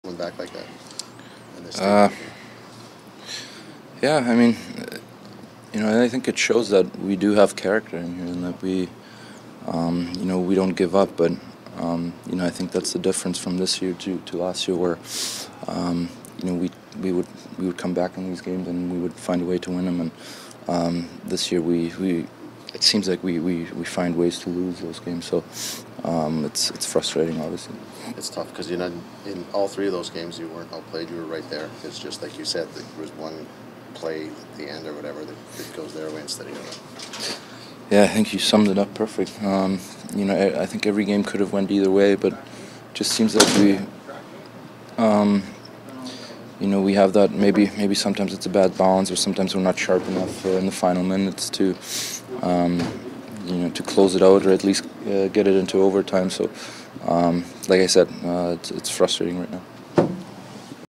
POST-GAME RAW | Leon Draisaitl
Leon speaks after the back and forth game in San Jose.